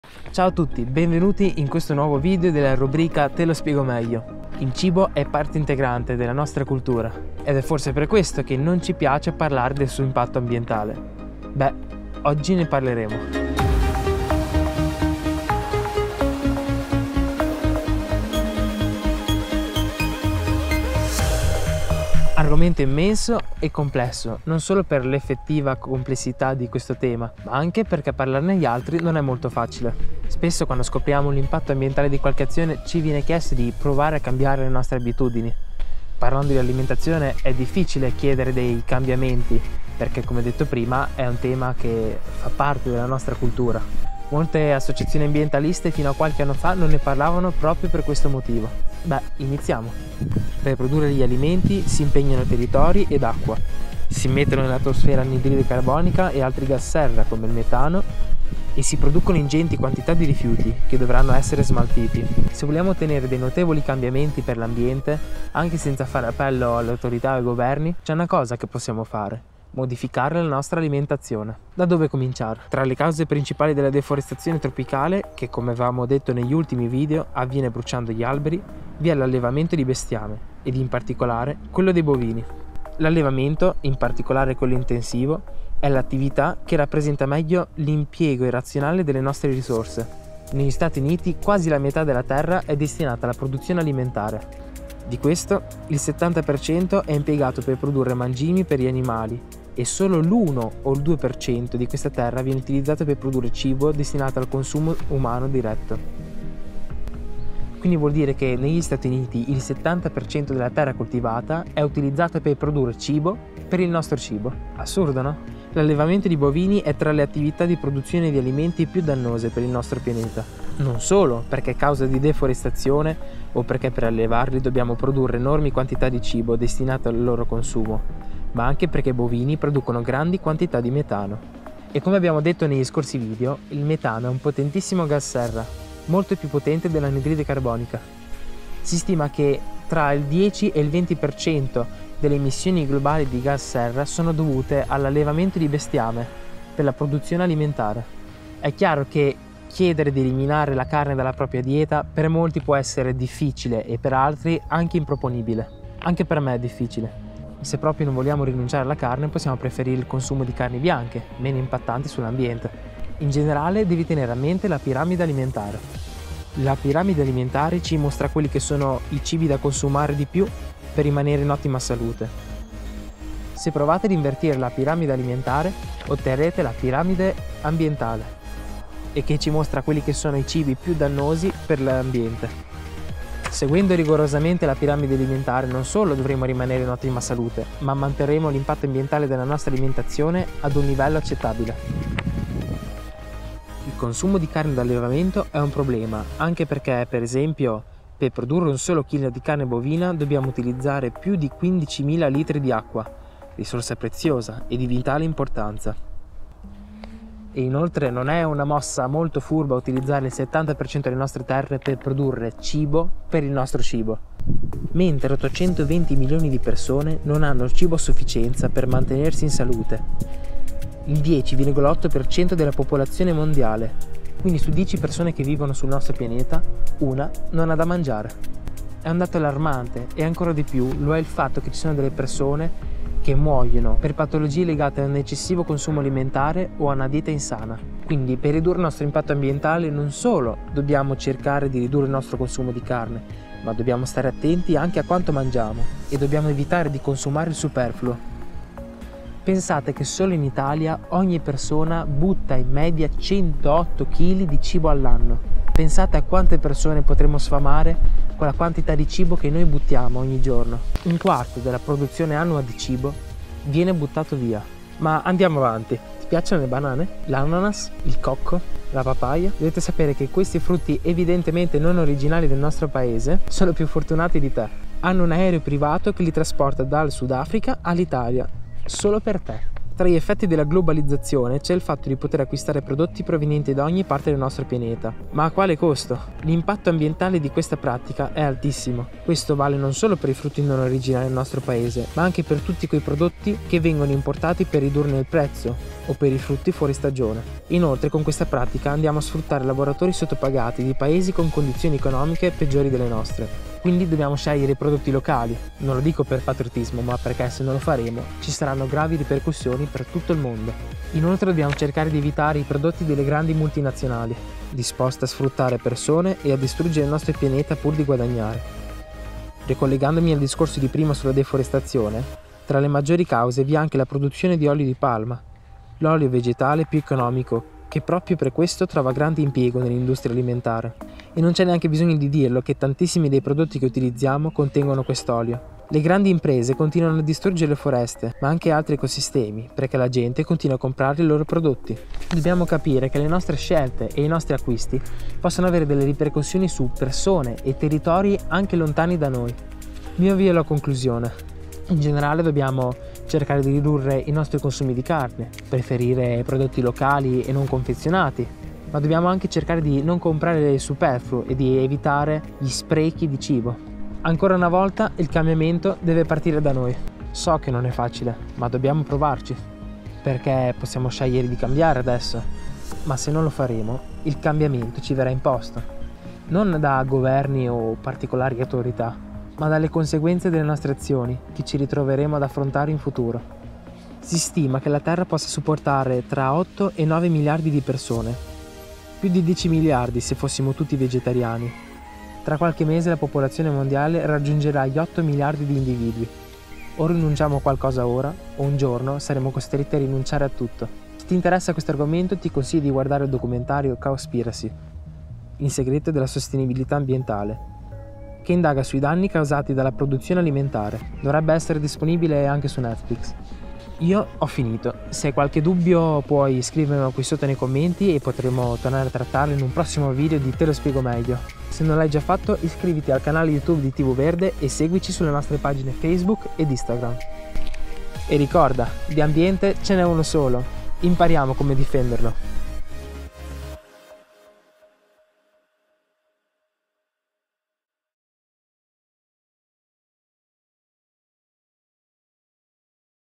Alimentazione e allevamento - Te lo spiego meglio
Il cibo fa parte della nostra cultura... Proprio per questo è difficile chiedere alle persone di cambiare. Ad ogni modo la produzione alimentare è tra le attività umane più dannose per l'ambiente.

Montaggio e riprese di Matteo Cimitan.